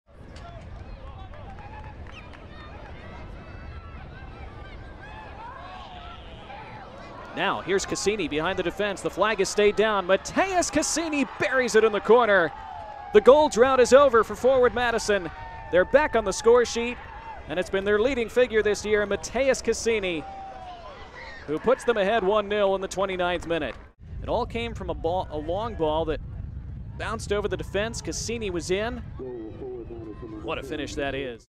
Matheus Henrique Cassini de Paula with a Goal vs. FC Tucson
Matheus Henrique Cassini de Paula (Forward Madison FC) with a Goal vs. FC Tucson, 10/15/2022